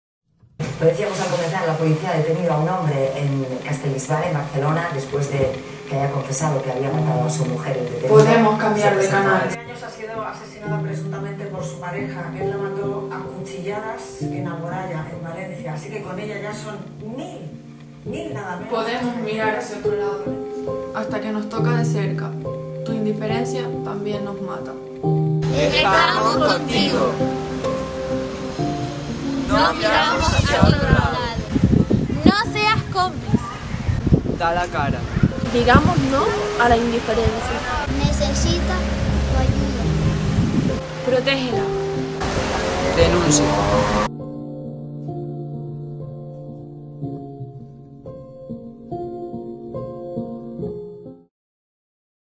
'No a la indiferencia'
'No a la indiferencia' es el spot realizado por un grupo de jóvenes de El Paso que consiguió el tercer premio en el II Concurso de Spots juveniles contra la Violencia de Género convocado por la Consejería de Acción Social, Diversidad, Igualdad y Juventud del Cabildo de La Palma.